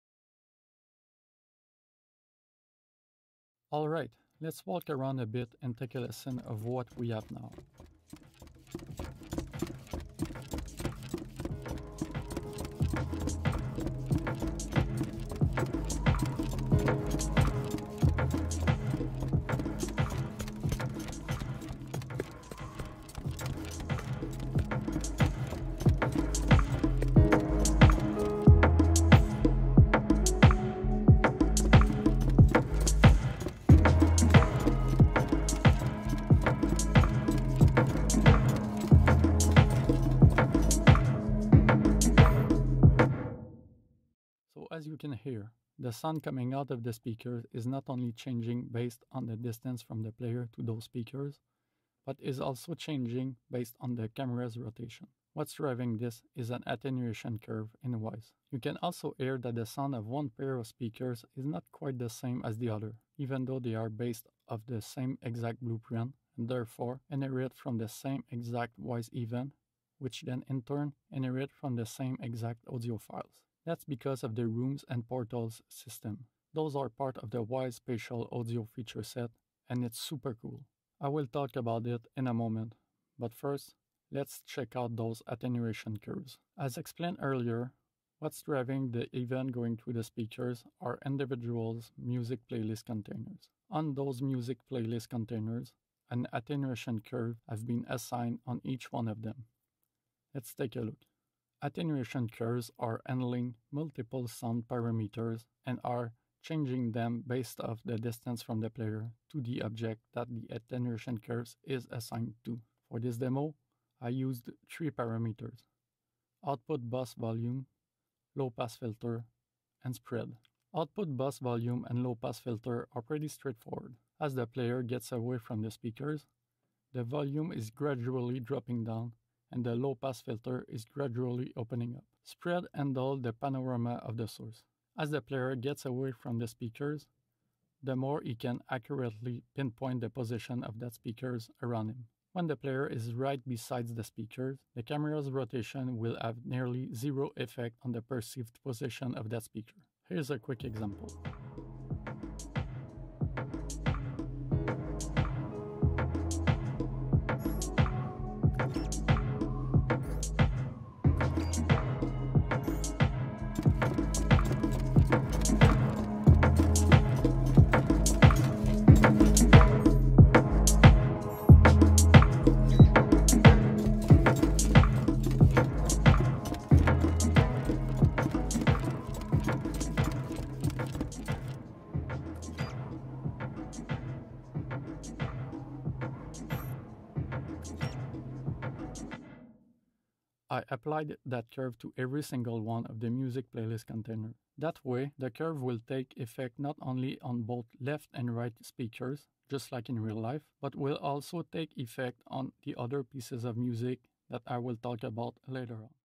Placing Sounds In 3D Spaces (Part. 1 - Attenuation Curves) - Wwise UE4 Integration 2020
In this video, I show you how Attenuation Curves are responsible of how the player perceives the speakers implemented in the last video.

0:00 Example
0:44 Explanation
1:42 Wwise Setup
2:30 Spread Example